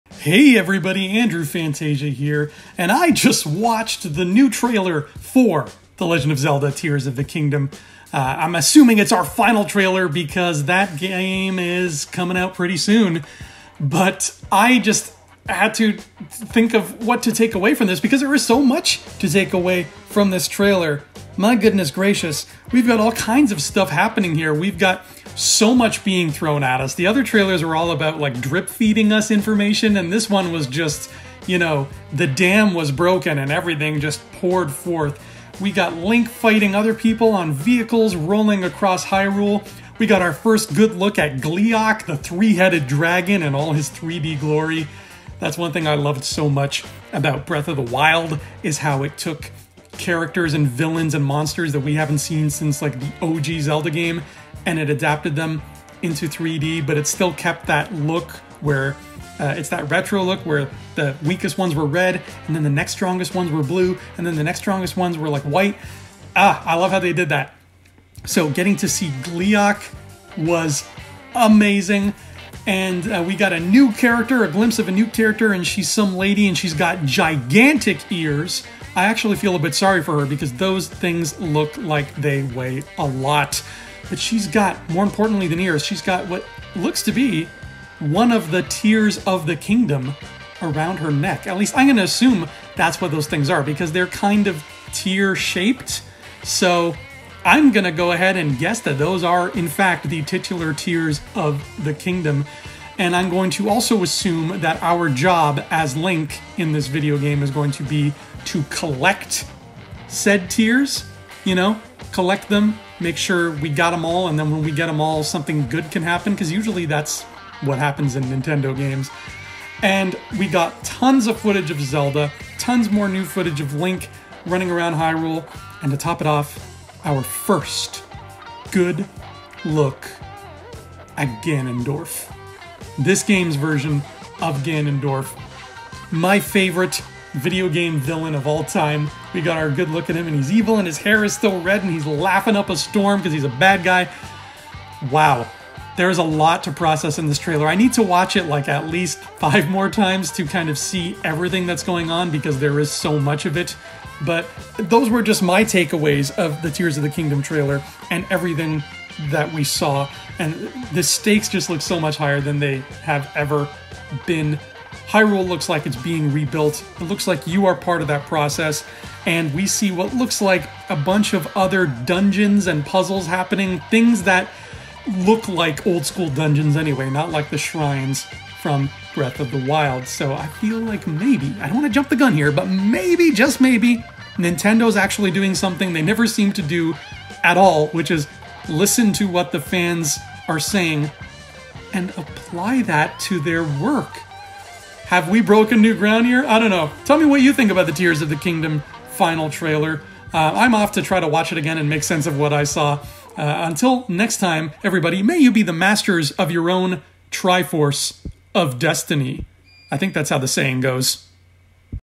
Legend of Zelda TEARS OF THE KINGDOM Final Trailer THOUGHTS
The Final trailer for the newest Zelda game looks amazing!